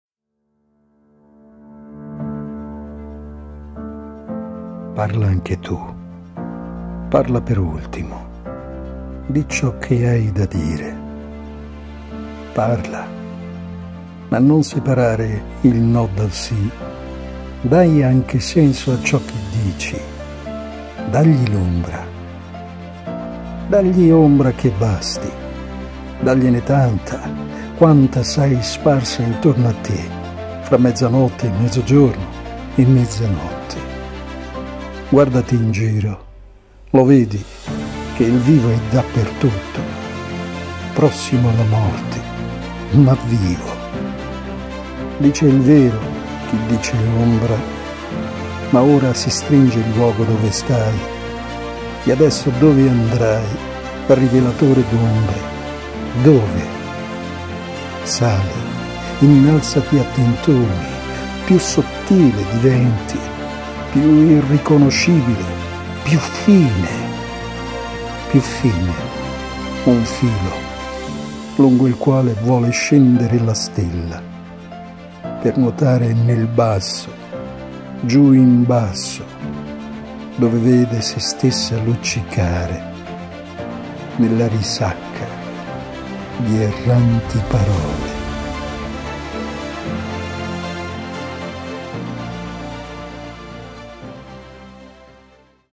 Paul Celan - Parla anche tu
PARLA ANCHE TU

Parla anche tu,

parla per ultimo,

di’ ciò che hai da dire.

Parla –

ma non separare il no dal sì.

Dai anche senso a ciò che dici:

dagli l’ombra.

Dagli ombra che basti,

dagliene tanta

quanta sai sparsa intorno a te

fra mezzanotte e mezzogiorno e mezzanotte.

Guardati in giro:

lo vedi, che il vivo è dappertutto –

Prossimo alla morte, ma vivo!

Dice il vero, chi dice ombra.

Ma ora si stringe il luogo dove stai:

e adesso dove andrai, rivelatore d’ombre, dove?

Sali. Innalzati a tentoni.

Più sottile diventi, più irriconoscibile, più fine!

Più fine: un filo,

lungo il quale vuole scendere la stella:

per nuotare nel basso, giù in basso

dove vede sè stessa luccicare: nella risacca

di erranti parole.